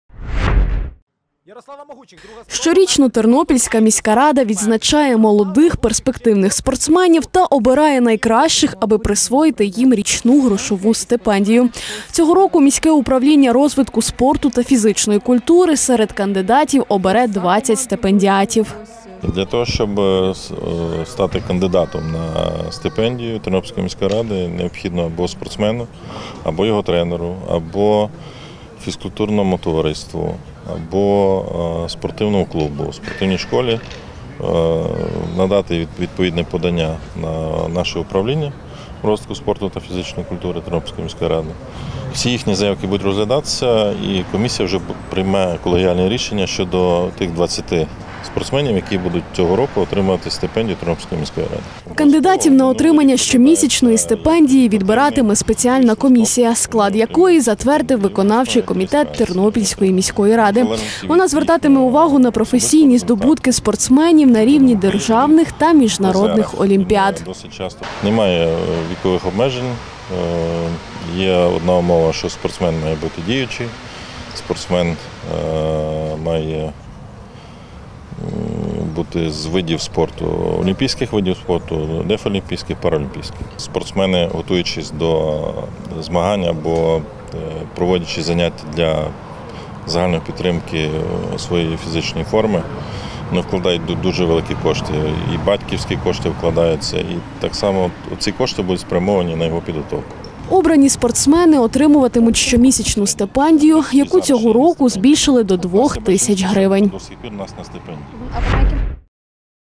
20 спортсменів отримають стипендію від Тернопільської міської ради
20 спортсменів отримають стипендію від Тернопільської міської ради. Щорічно спеціальна комісія обирає найкращих кандидатів Тернополя та відзначає їх щомісячною грошовою винагородою. Хто може стати учасником конкурсу - далі в сюжеті.